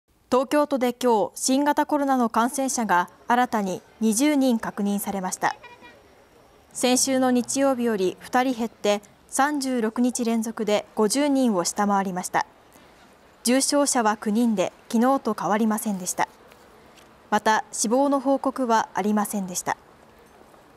東京で新たに20人の感染確認（2021年11月21日）
東京都できょう、新型コロナの感染者が新たに20人確認されました。
先週の日曜日より２人減って36日連続で50人を下回りました。
重症者は、９人できのうと変わりませんでした。
また、死亡の報告はありませんでした。